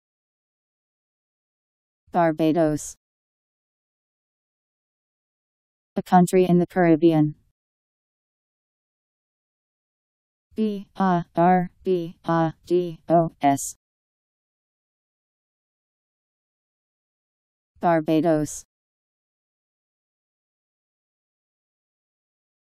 Barbados Meaning
Video shows what Barbados means. A country in the Caribbean..  Barbados pronunciation. How to pronounce, definition by Wiktionary dictionary. Barbados meaning. Powered by MaryTTS